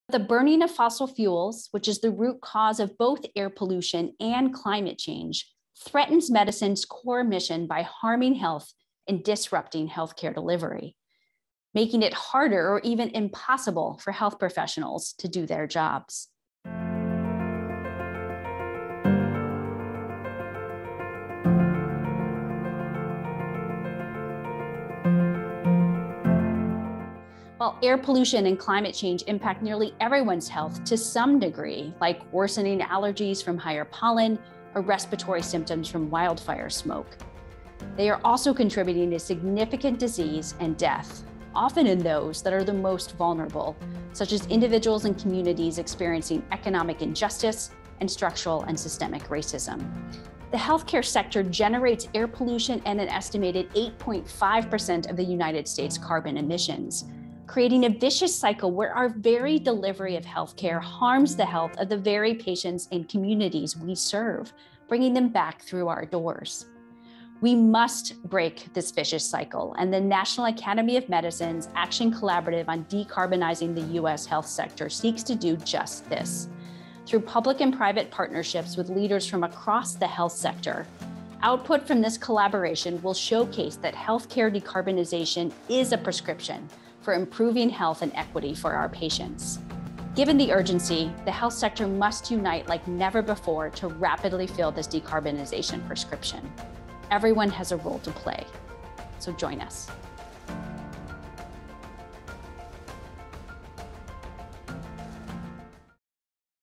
Health Care Leaders Speak Out on Climate Change - Renee Salas
Renee N. Salas, MD, MPH, MS, Affiliated Faculty / Yerby Fellow, Harvard Global Health Institute / Harvard T.H. Chan School of Public Health: "The burning of fossil fuels, which is the root cause of both air pollution and climate change, threatens medicine’s core mission by harming health and disrupting health care delivery, making it harder or even impossible for health professionals to do their jobs." Hear more leaders speak out on climate change